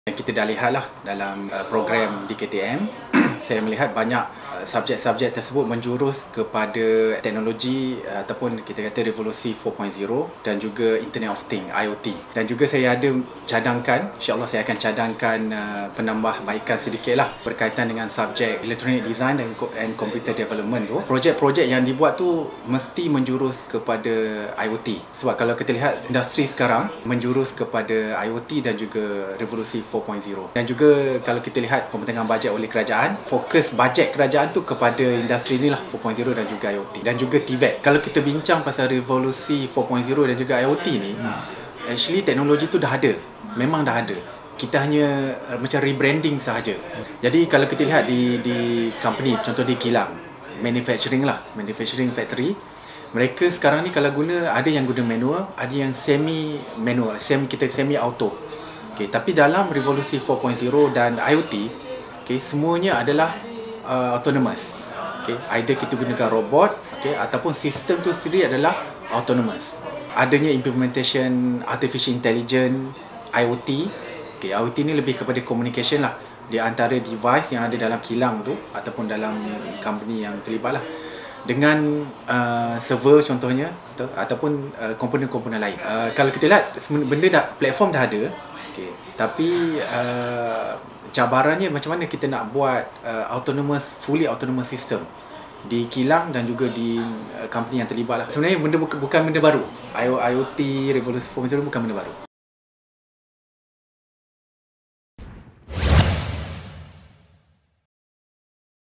DIP KEJURUTERAAN TEKNOLOGI MULTIMEDIA KOLEJ UNITI KE ARAH REVOLUSI PERINDUSTRIAN IR 4 0
Diploma Kejuruteraan Teknologi Multimedia (DKTM) di bawah Fakulti Teknologi Kejuruteraan Kolej Uniti adalah salah satu diploma kolej-kolej bersekutu Universiti Putra Malaysia.

Video kali ini memaparkan temuramah External Assessor DKTM berkaitan dengan keperluan pelajar DKTM dalam menghadapi cabaran Revolusi Industri 4.0.